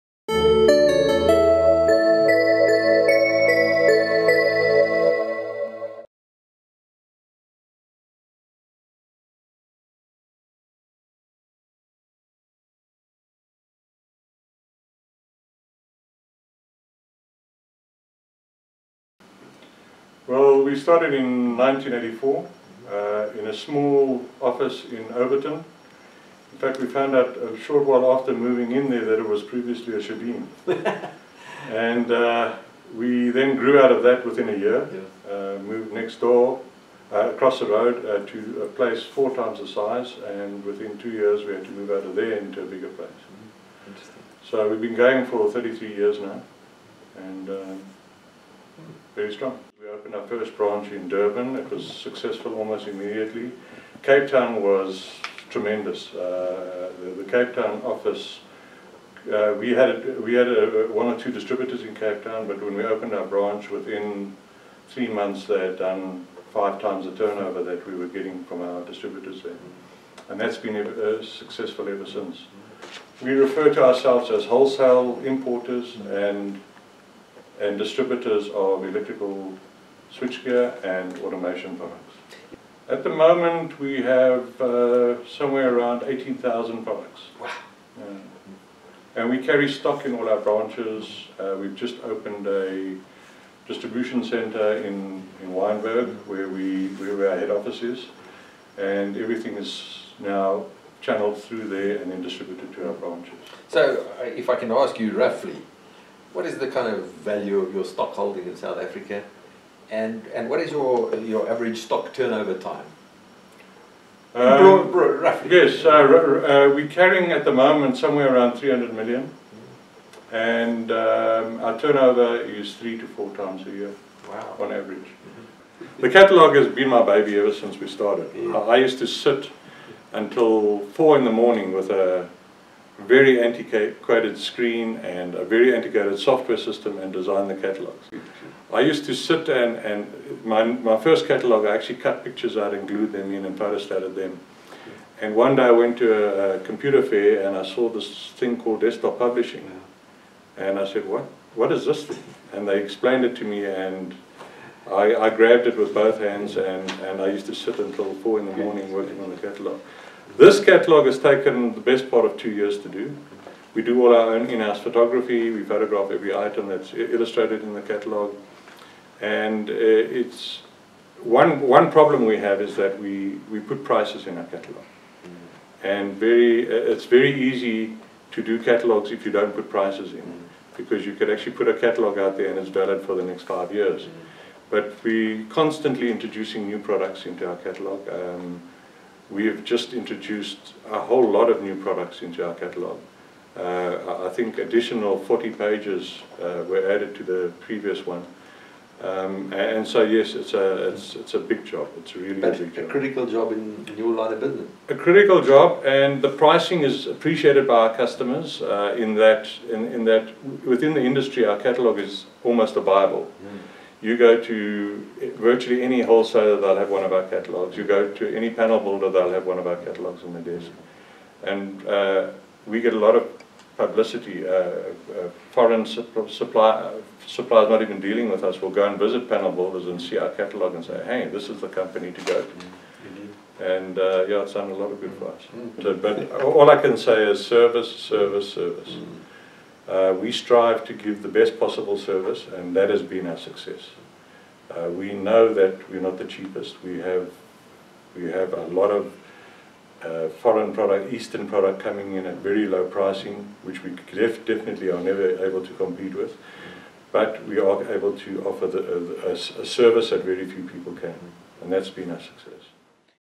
David van den Berg on ElectroMechanica's successes and new catalogue
David van den Berg, managing director of ElectroMechanica, speaks to EE Publishers’ Chris Yelland about his company’s successes and its new 2017 catalogue.